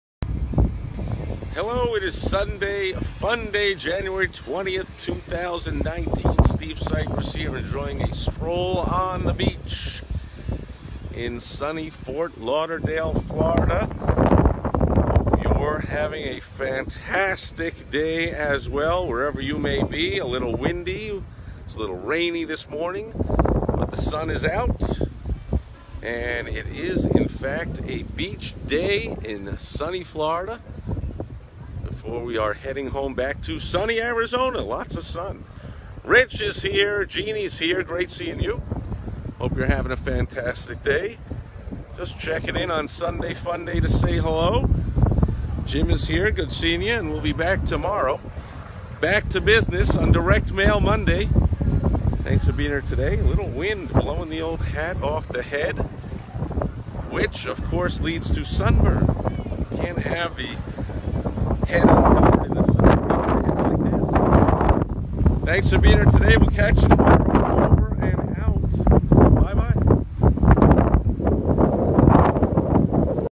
Sunday Funday - Life’s A Beach
Enjoying one last day in sunny Florida before heading home.